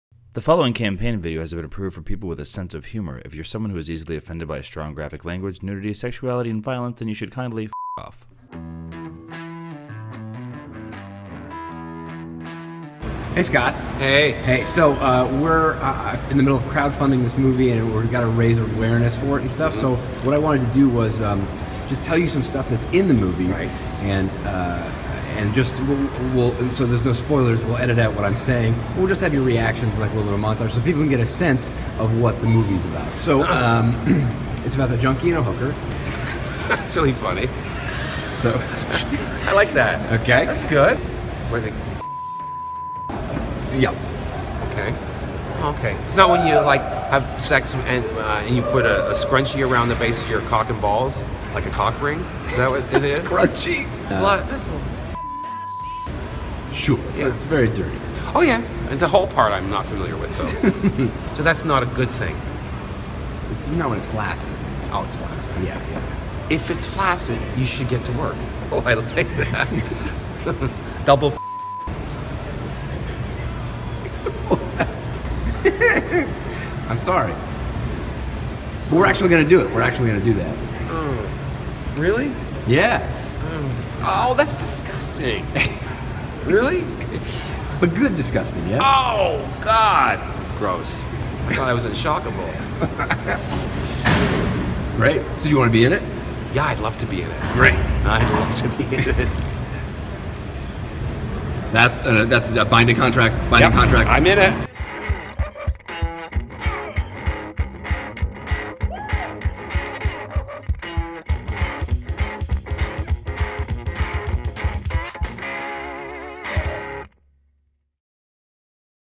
GO-GETTERS: Spitballin' with Scott Thompson
Scott Thompson reacts to Aaron Abrams telling him about the upcoming film The Go-Getters (with all the details cut out).  Please help us make the film by contributing